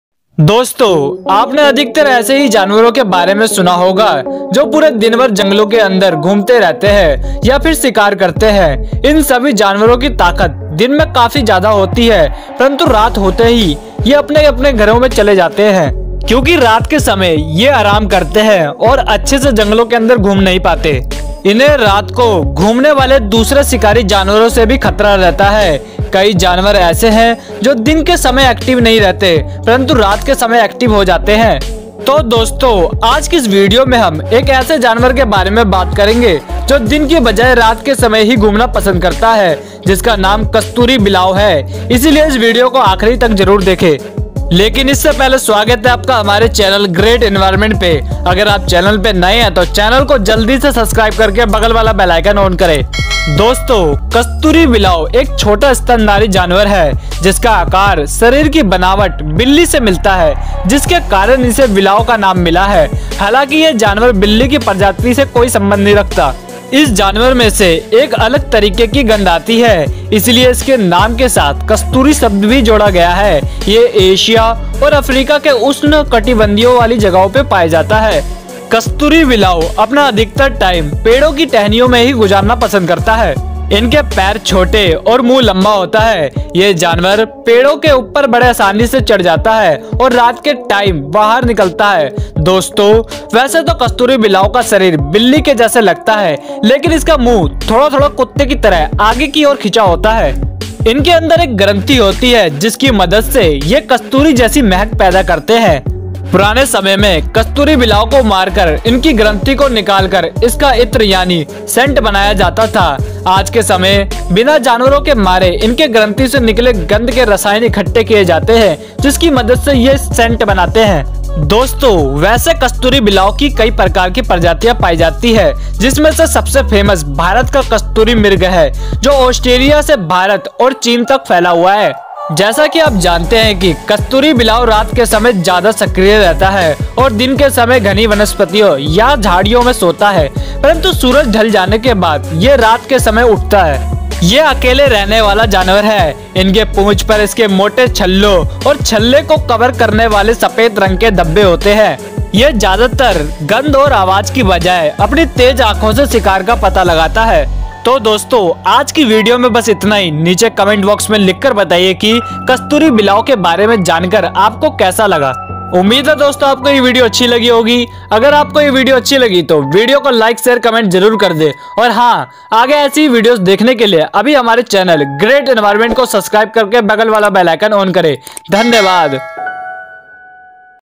ऐसा जीव जो दिन की बजाय रात को शिकार करता है - Information About Civet - [Hindi] - Great Environment
Information About Civet - [Hindi] - Great Environment. दोस्तों आज की इस वीडियो में हमने कस्तूरी बिलाव के बारे में बताया है जिसमें  कस्तूरी बिलाव से जुड़ी कुछ रोचक तथ्य भी है। दोस्तों कस्तूरी बिलाव एक ऐसा जानवर है जो रात के समय ज्यादा सक्रिय रहता है।

Friends, in this video of today, we have told about Civet, which also has some interesting facts related to Civet. Friends, the musk tomcat is an animal that is more active during the night.